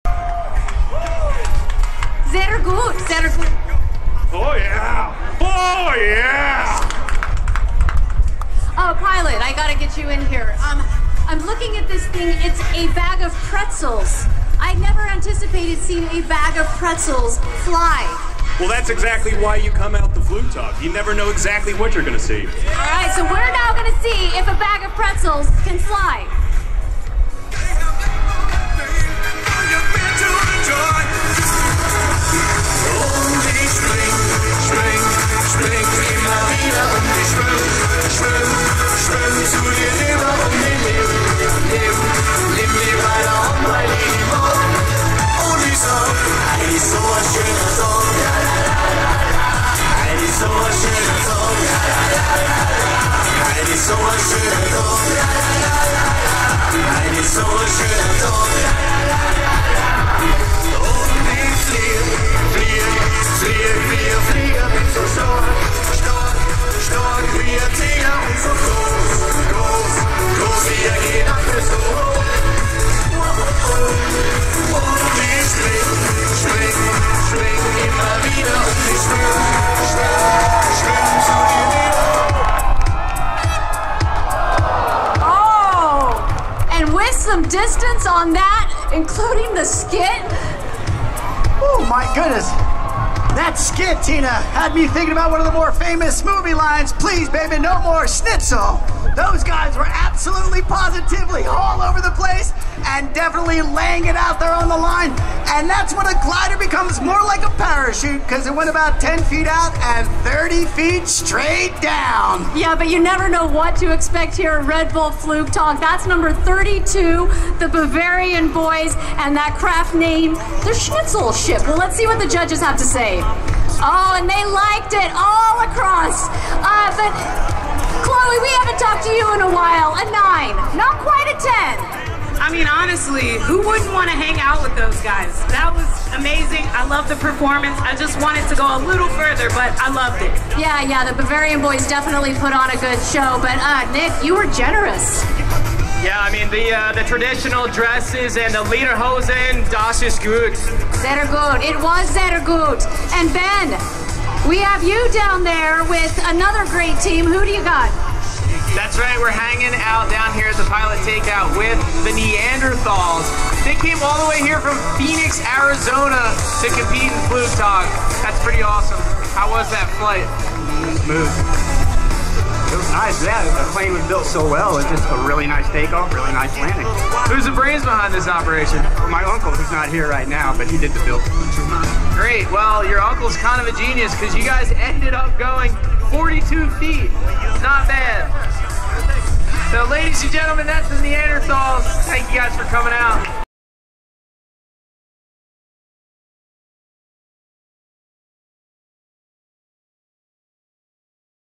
From Pretzels to Flight: The Unforgettable Journey of the Bavarian Boys at Red Bull Flugtag!
85+ live channels - No contracts or hidden fees.